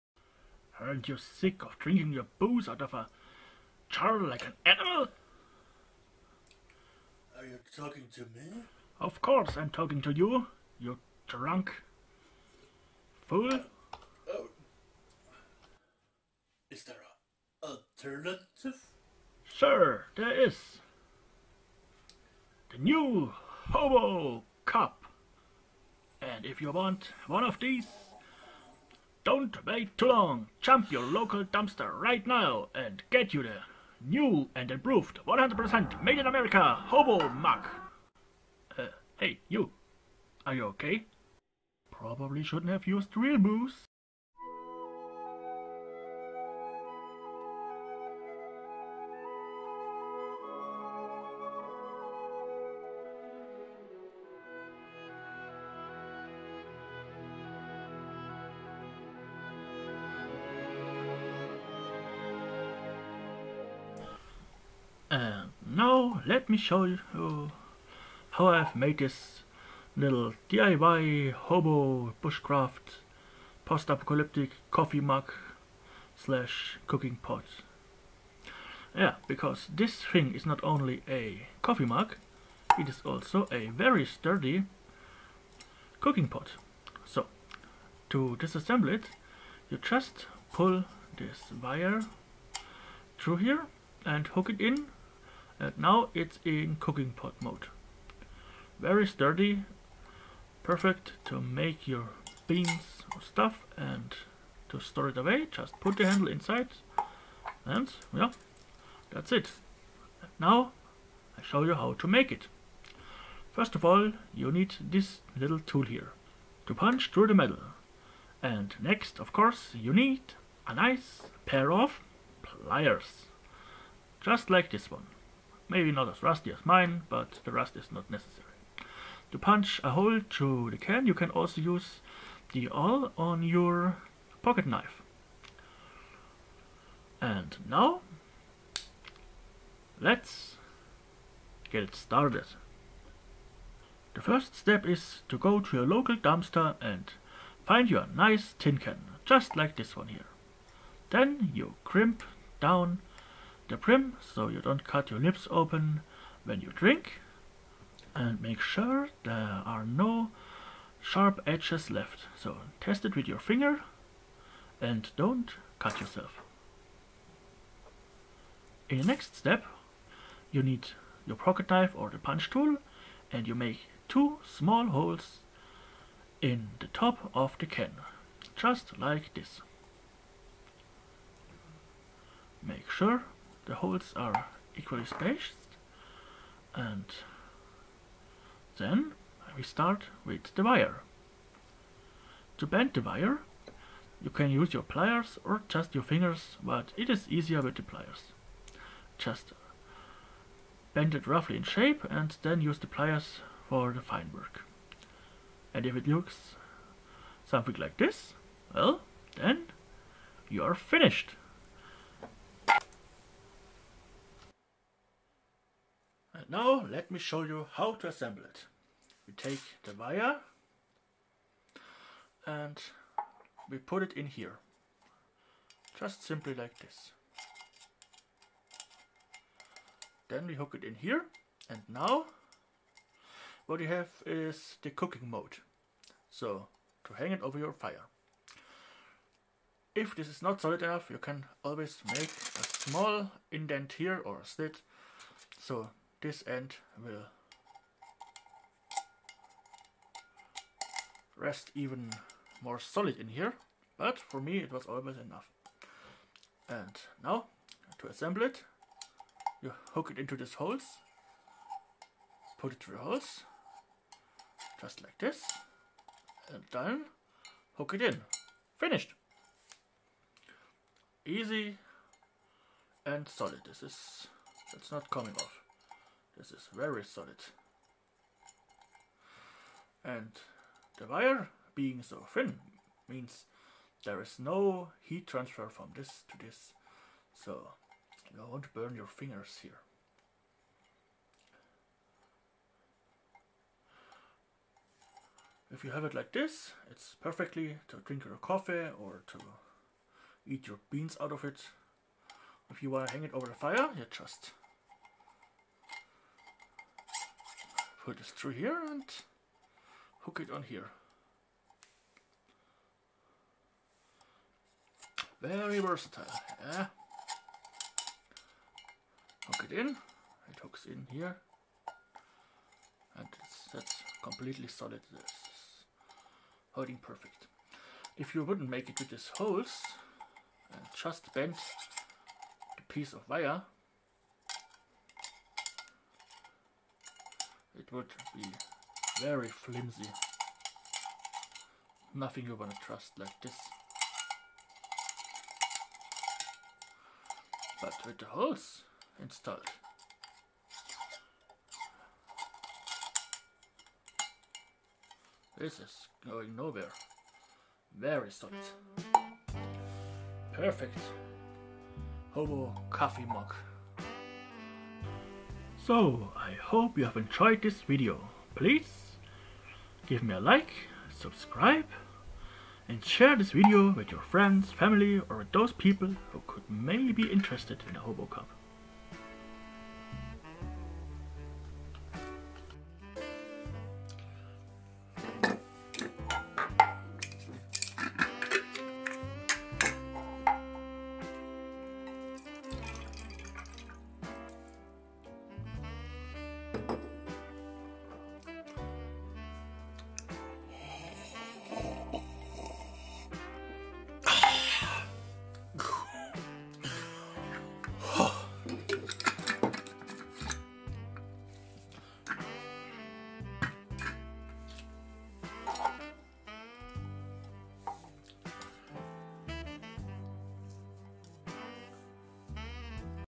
Hobo Style Tin Can Coffee Mug / Cooking Pot [Easy DIY]
Have you ever been camping outside in nature and didn't want anything more than a nice, hot cup of coffee / tea? Then my little invention could help you next time!

Let me know if you liked the idea and share the video with me if you recreate it :-)